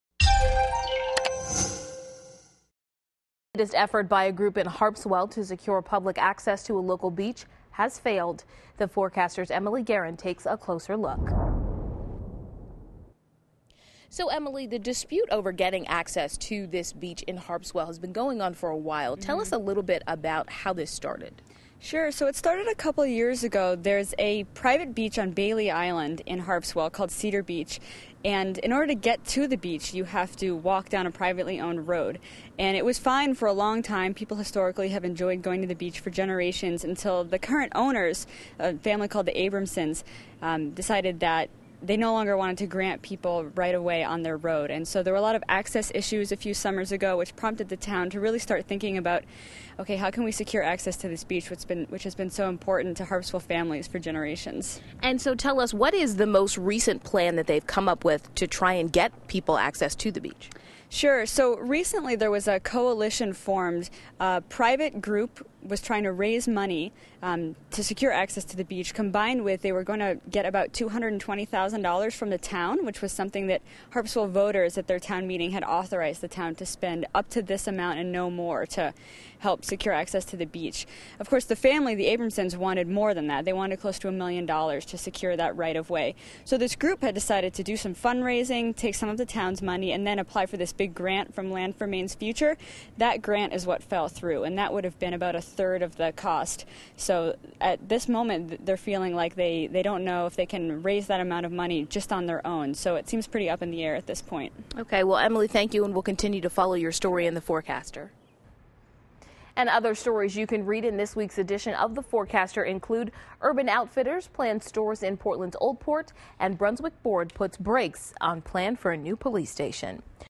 Forecaster Reporter Discusses Harpswell Beach Access Update
Forecaster reporter Emily Guerin talks to News 8's Thema Ponton about the latest development in the controversy surrounding beach access to Cedar Beach on Bailey Island